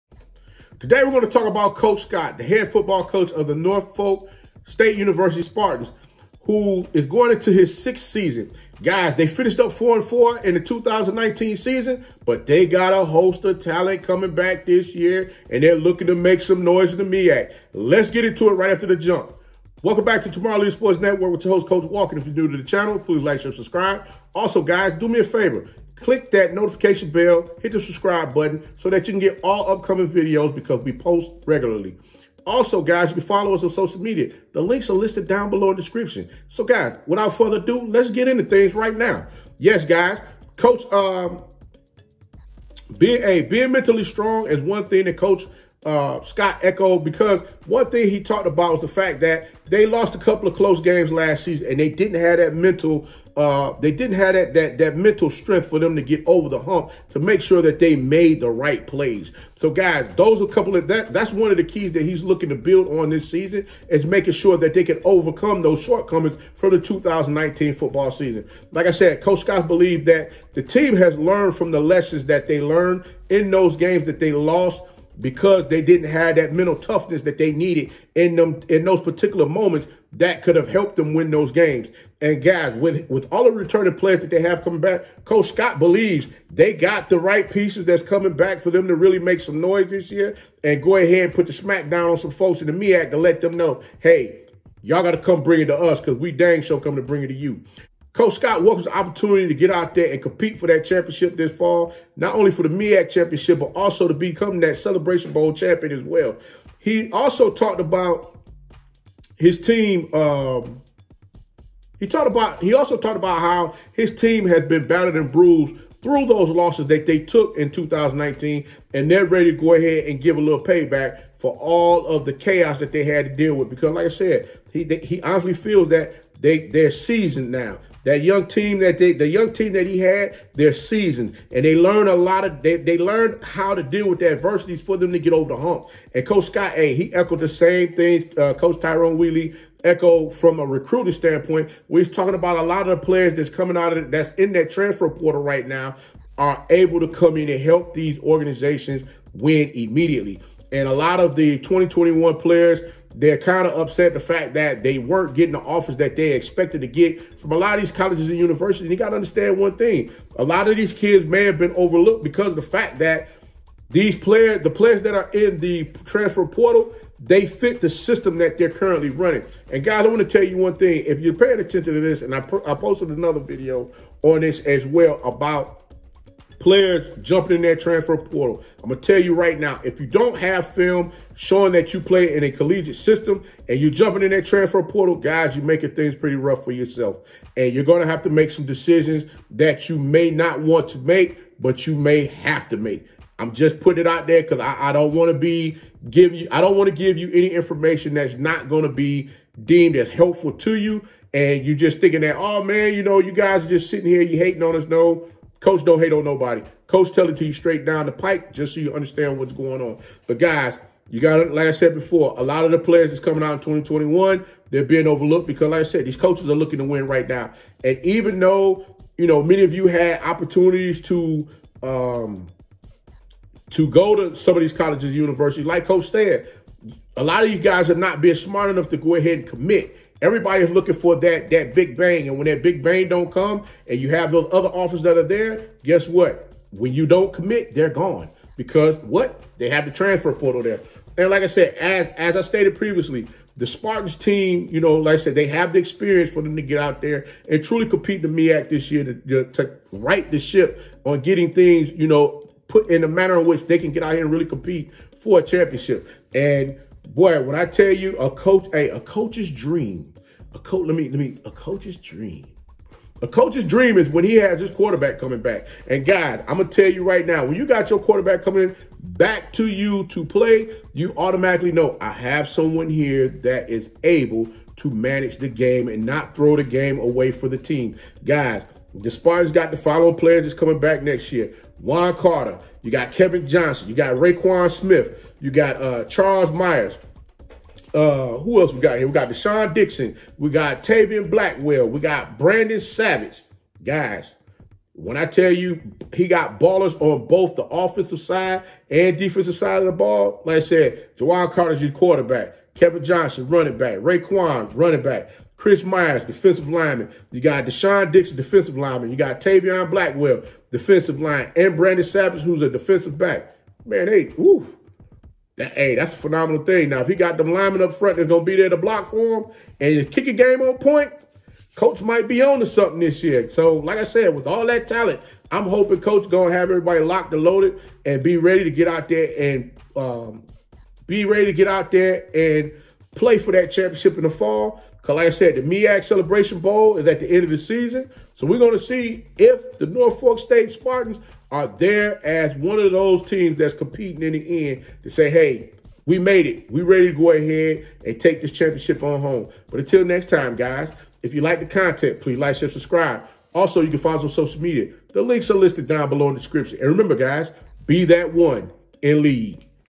Norfolk State University Football Coach Latrell Scott | 2021 MEAC Media Day
In this episode, Coach Walker talks about Norfolks State University MEAC Media Day.

Mailing Address:👇🏾
Tomorrow's Leaders Sports Network
863 Flat Shoals Rd